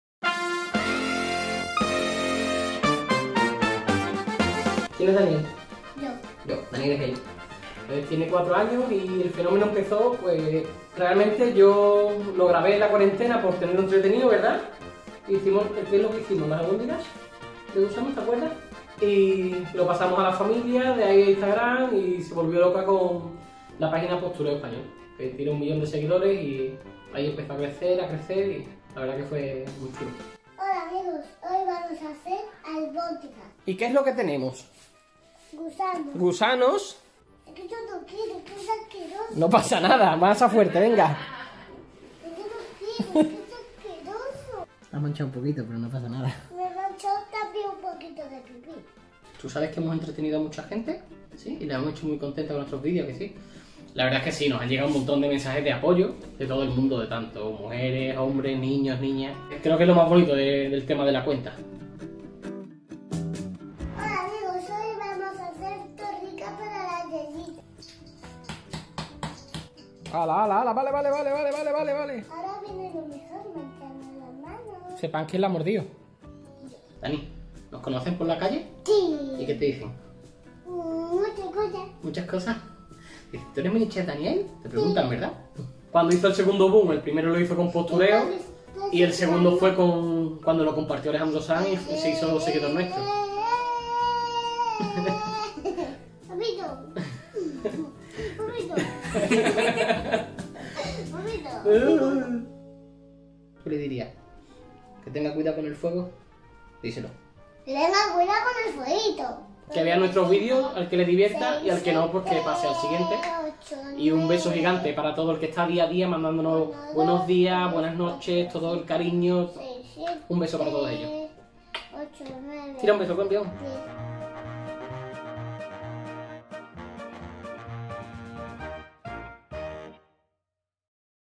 MiniChef Dani: "Nuestra cuenta empezó a crecer y hasta el día de hoy"
Entre sus recetas más famosas encontramos las ‘Albóndigas de gusanos’, las ‘Magdalenas de Sangre’ o las ‘caquitas de unicornio’. Su padre Israel y administrador de la cuenta, confiesa que “no sabemos hasta cuando tendremos vídeos de MiniChef, hasta que Dani diga ‘basta'.